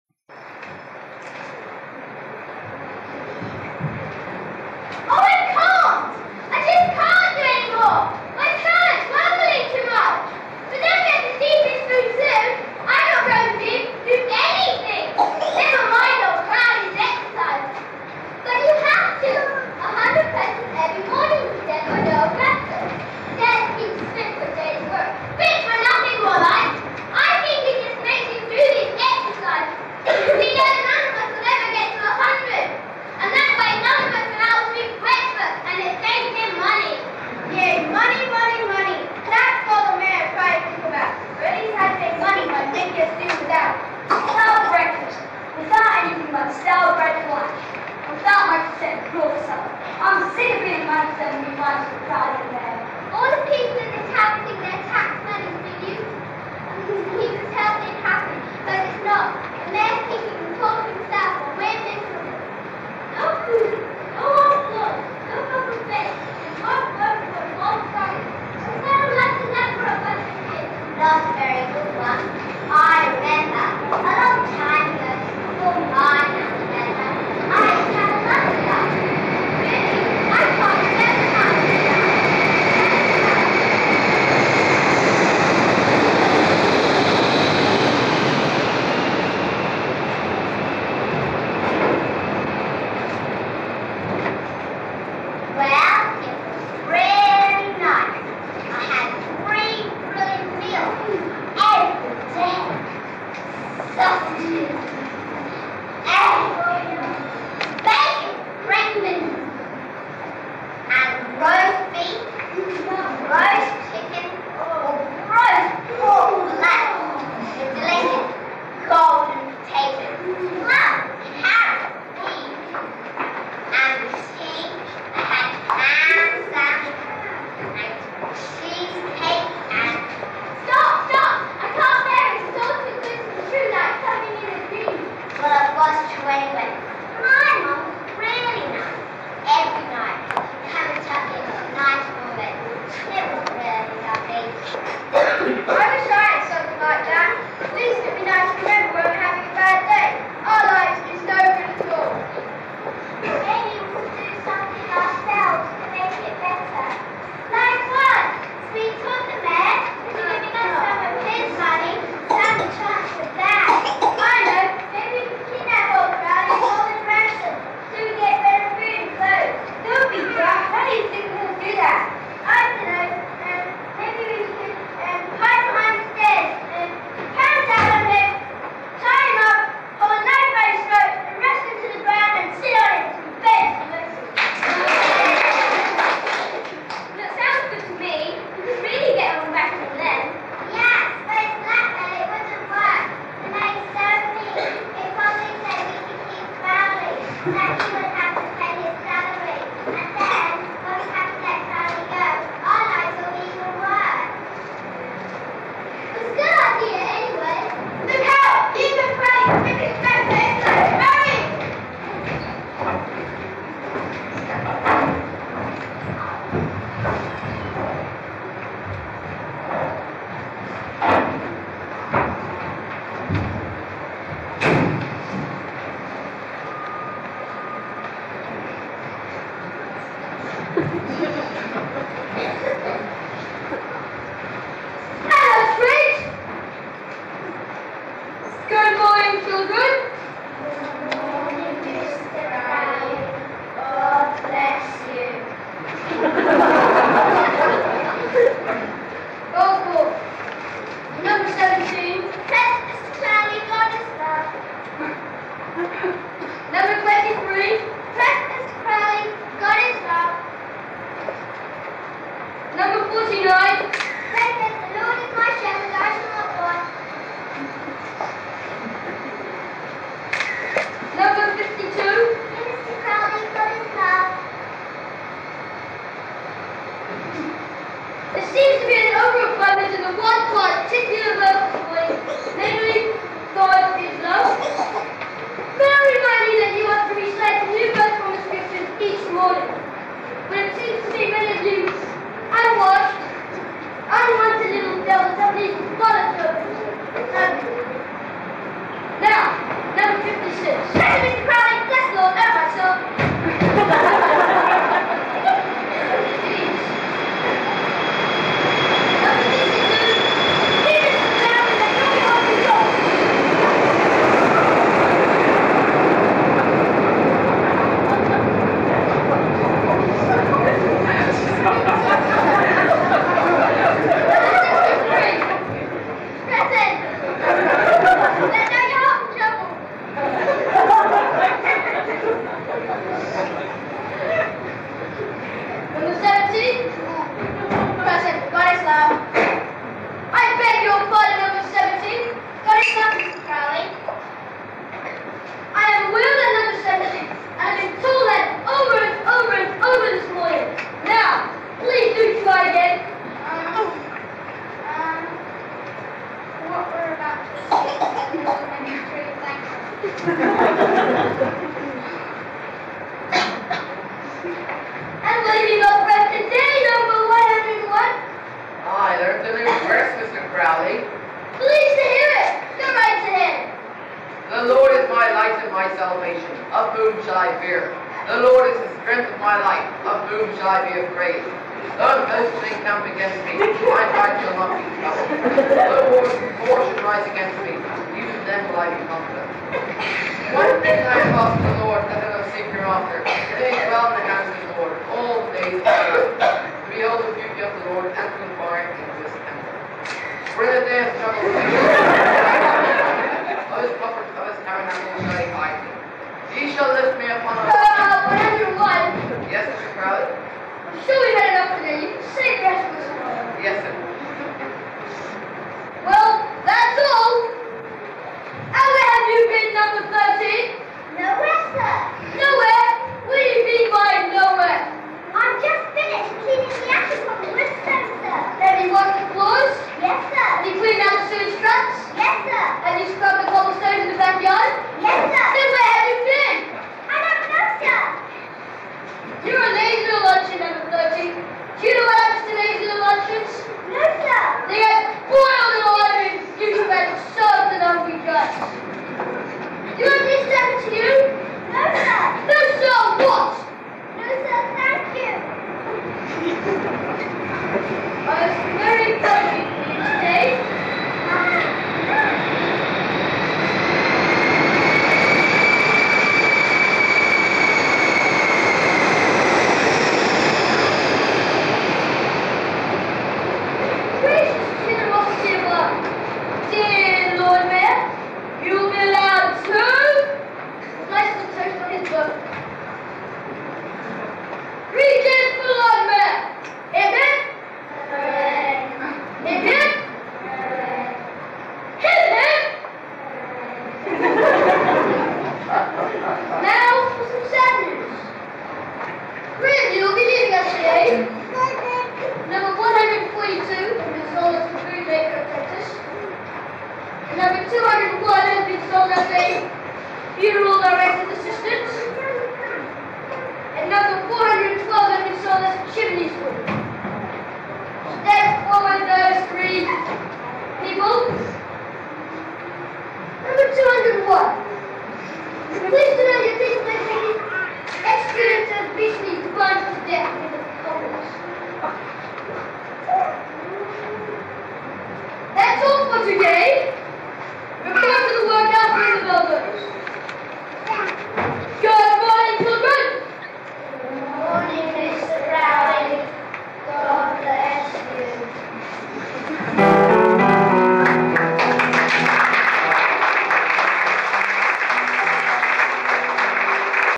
The Piperman Part 2
Boundary Junior School (BJS) circa 1985 production of The Piperman.